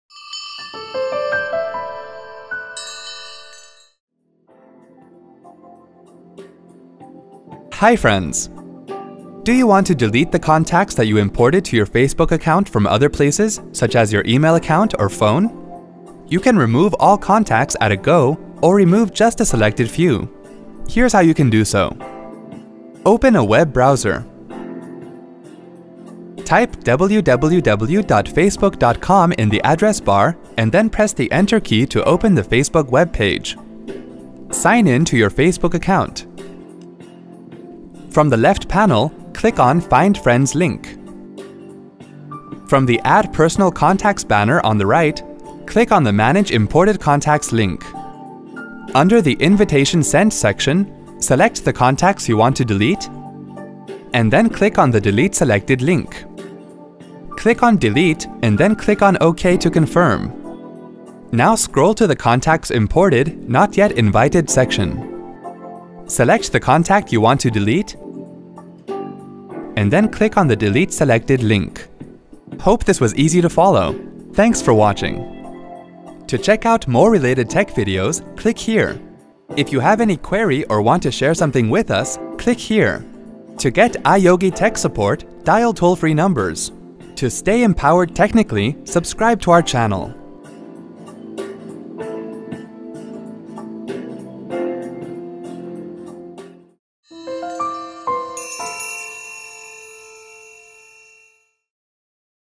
How to delete imported contacts on Facebook® :Tutorial
Do you want to delete the contacts that you imported to your Facebook® account from other places such as your email account or phone? Watch this video and follow the steps to do so.